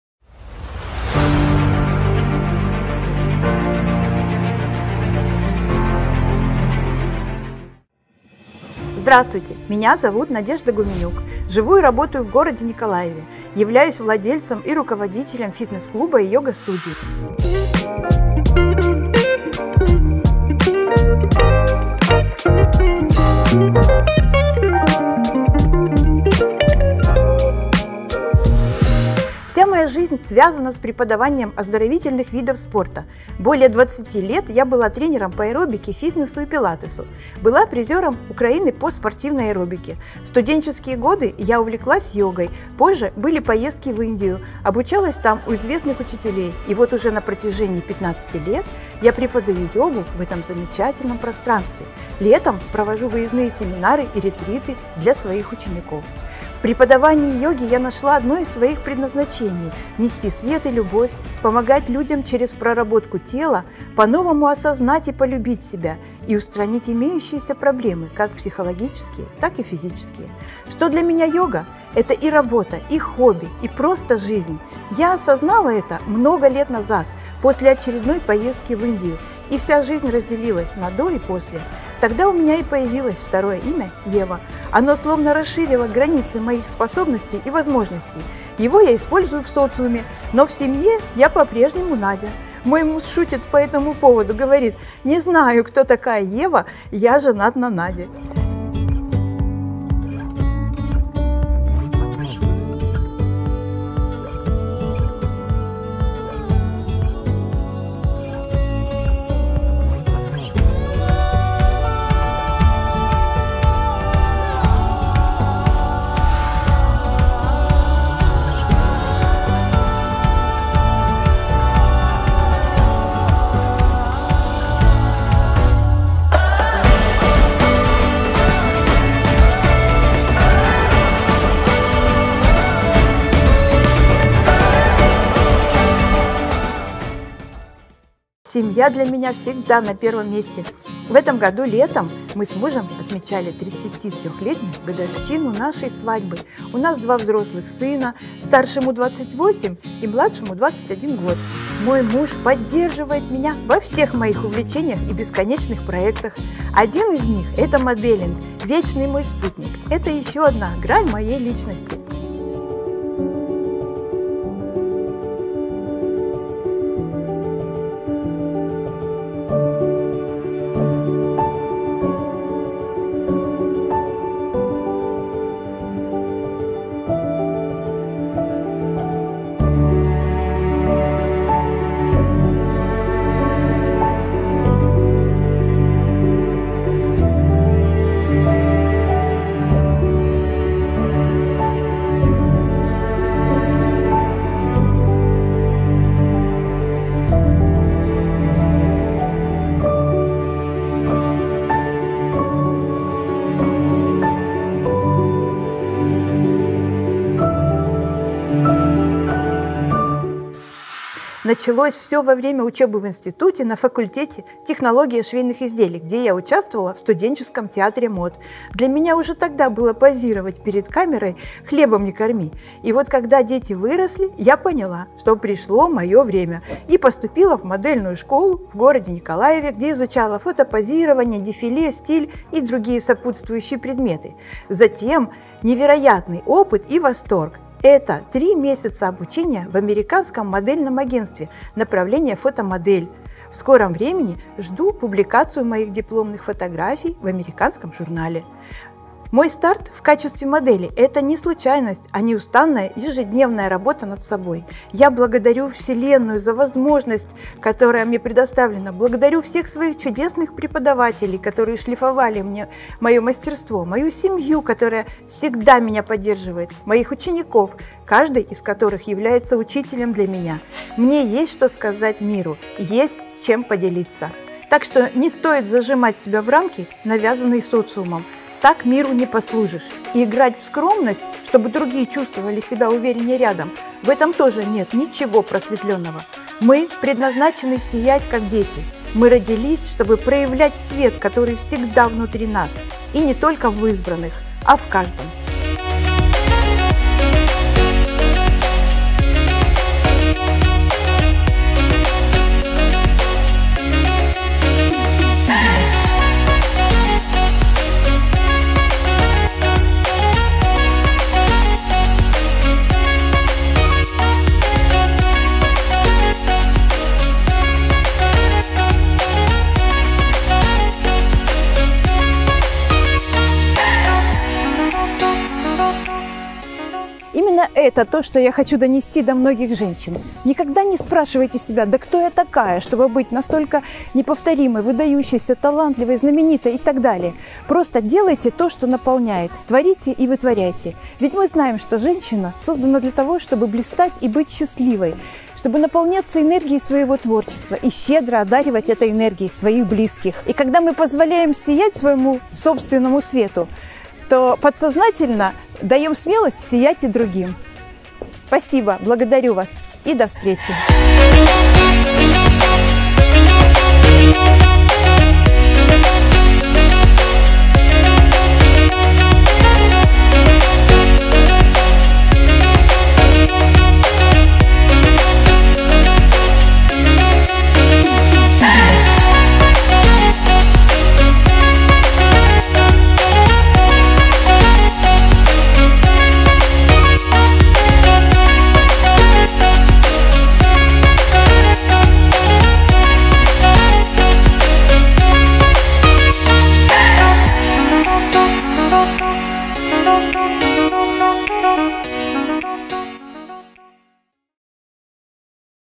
Golden Time Talent | GRAND STAR | Nadiia Humeniuk | GTGR-8505-3205
Имя участника или коллектив: Nadiia Humeniuk

Страна: Ukraine

Город: Nikolaev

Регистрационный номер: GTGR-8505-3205

Возрастная категория: от 50

GRAND STAR

Участие в других конкурсах и фестивалях / творческие достижения: Меня зовут Надежда Гуменюк , живу и работаю в городе Николаеве, являюсь владельцем и руководителем фитнес-клуба и йога-студии. Вся моя жизнь связана с преподаванием оздоровительных видов спорта. Более 20 лет я была тренером по аэробике, фитнесу и пилатесу. Была призером Украины по спортивной аэробике. В студенческие годы я увлеклась йогой, позже были поездки в Индию, обучалась там у известных учителей и вот уже на протяжении 15 лет я преподаю йогу в моей студии, а летом провожу выездные семинары и ретриты для своих учеников.
В преподавании йоги я нашла одно из своих предназначений, нести свет и любовь, помогать людям через проработку тела по-новому осознать и полюбить себя и устранить имеющиеся проблемы - как психологические так и физические.
Йога для меня и работа, и хобби и просто жизнь! Семья для меня всегда на первом месте! В этом году летом мы с мужем отмечали 33 -летнюю годовщину нашей свадьбы!
У нас 2 взрослых сына - старшему-28 и младшему 21 год. Мой муж поддерживает меня во всех моих увлечениях и бесконечных проектах. Один из них это моделинг - вечный мой спутник и ещё одна грань моей личности. Началось это увлечение во время учебы в институте на факультете «технология швейных изделий», где я участвовала в студенческом театре мод.
И вот, когда дети выросли, я поняла, что пришло моё время